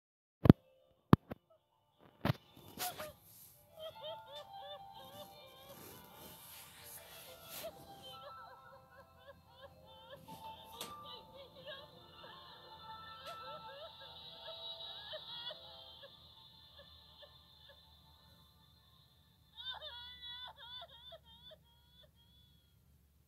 My reaction that windows 7 is ending support next year (2020)
NOOOOOOOOOOOOOOOOOO! I will miss you windows 7!
Note: I like sailor moon
If you hate sailor moon,I respect your opinion.